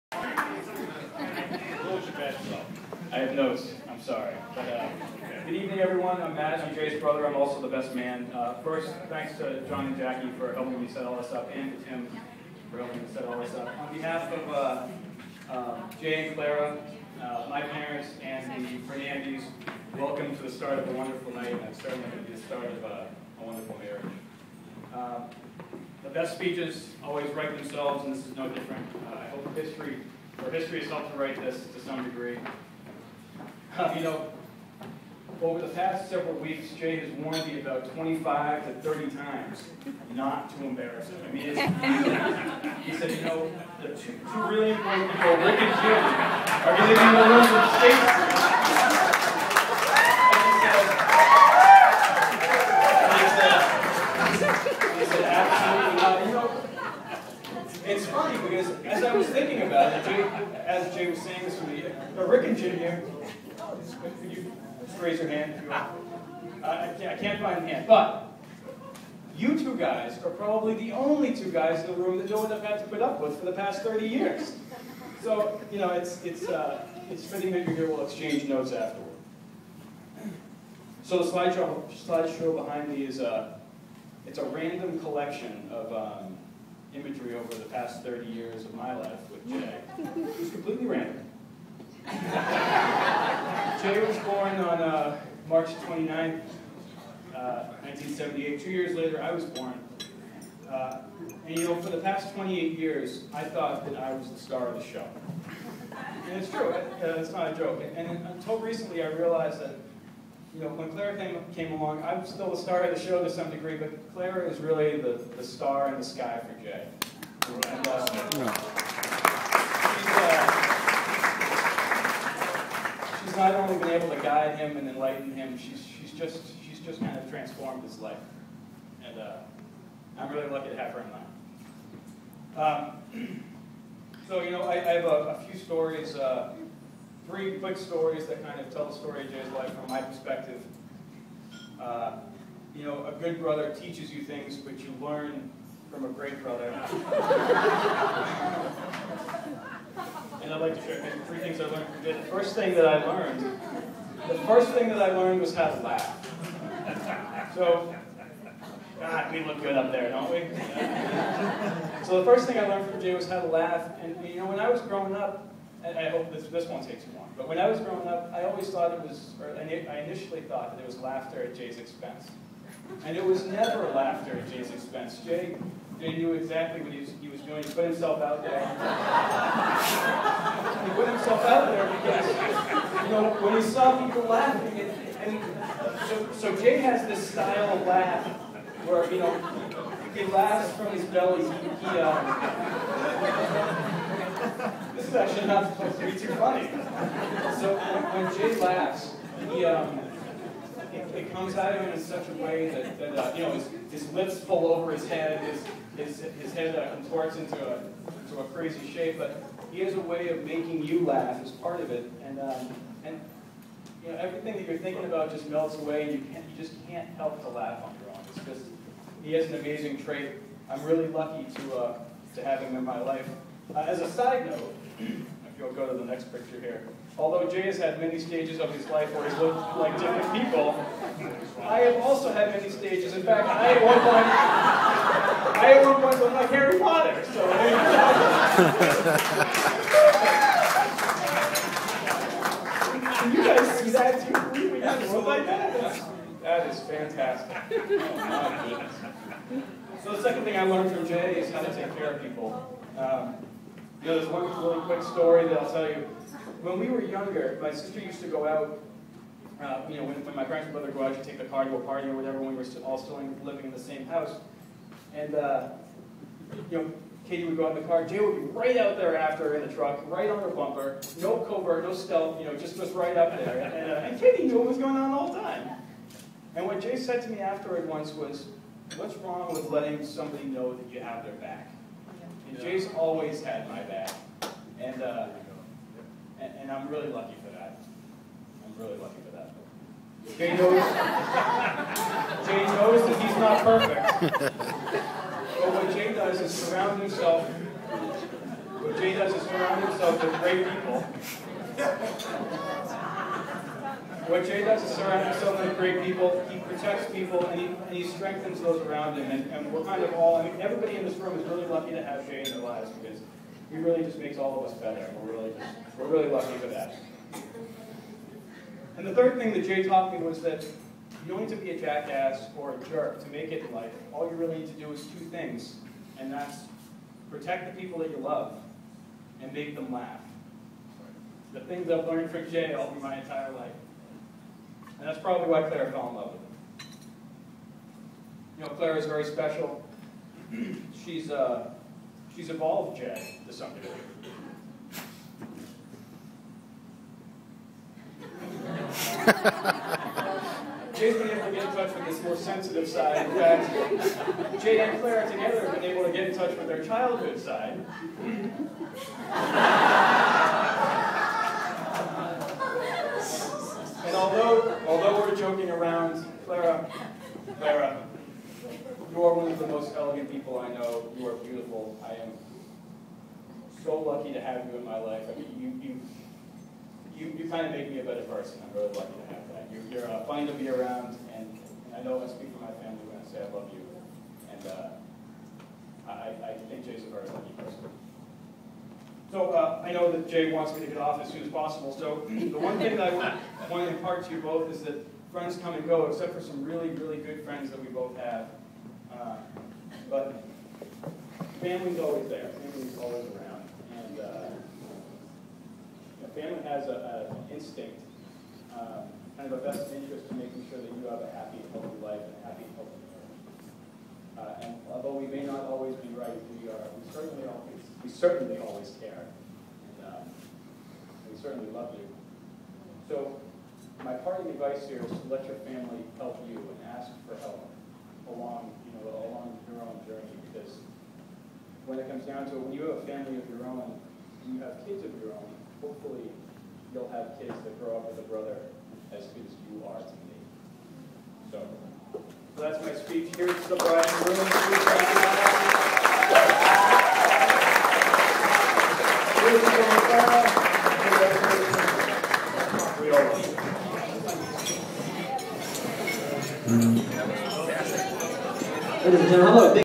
Jay & Clara Fulchino Wedding - 7/16/2011 - Best Man Speech
Jay's brother Matt gives a hilarious yet touching tribute.